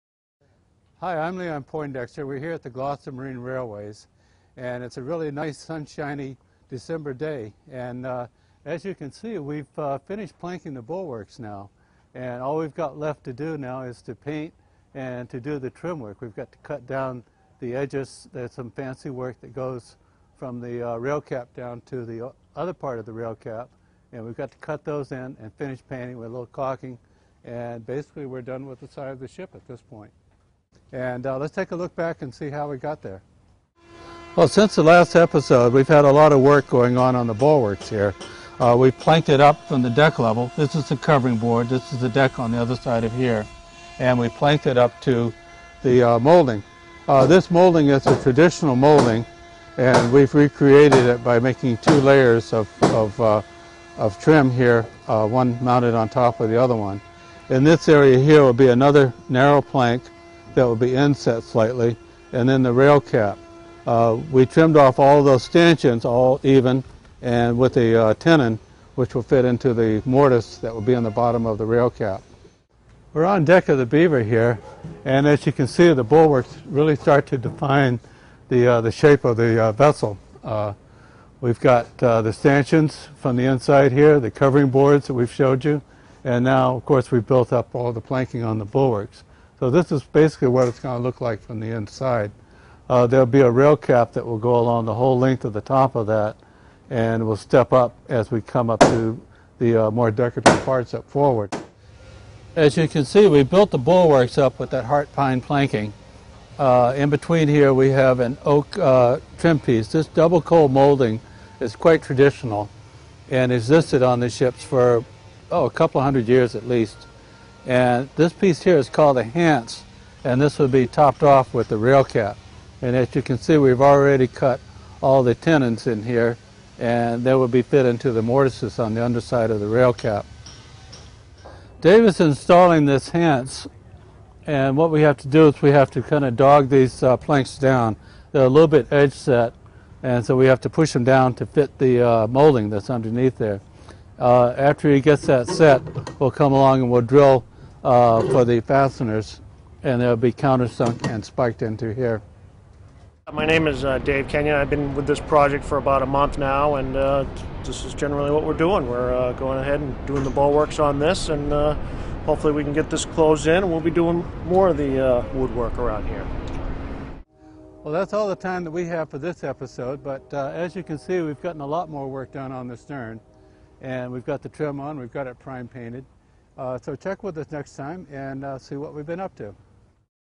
Boston Tea Party Ships & Museum: Bulwarks Are Done
Installing the bulwarks has been a defining step in overall look of the Beaver.